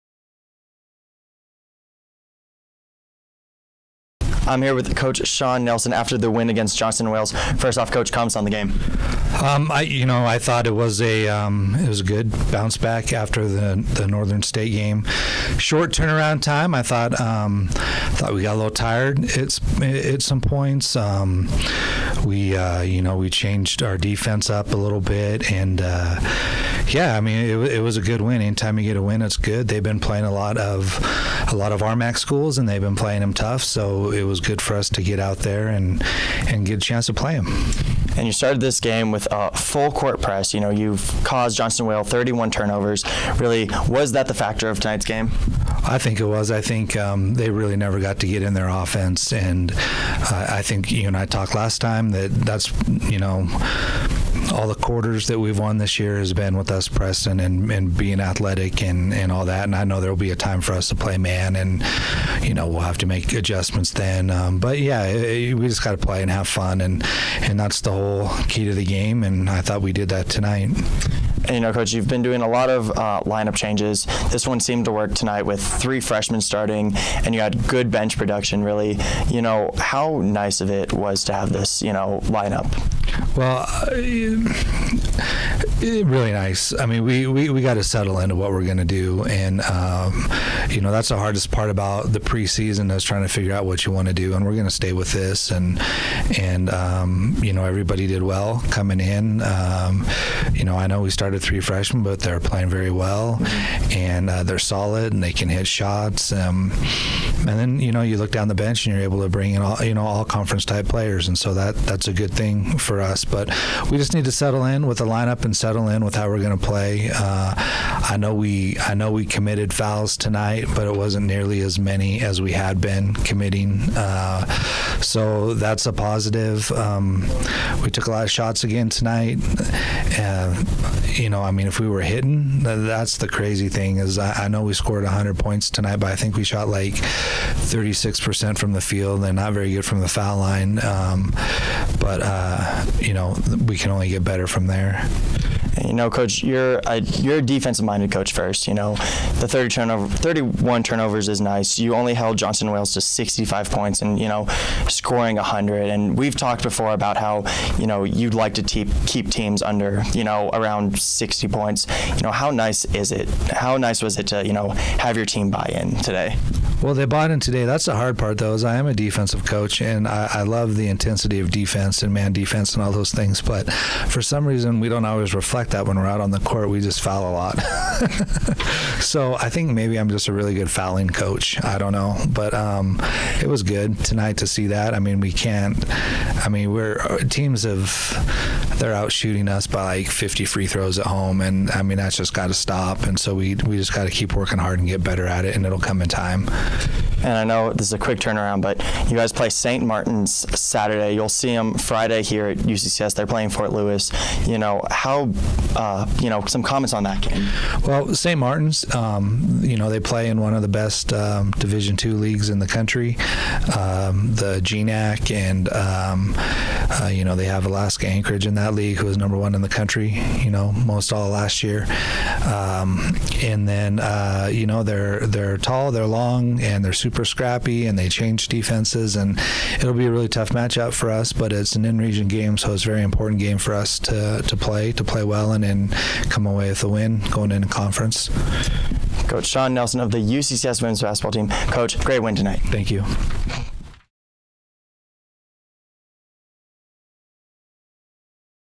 Shawn Nelson Postgame Johnson and Wales
Shawn Nelson talks about the win against Johnson and Wales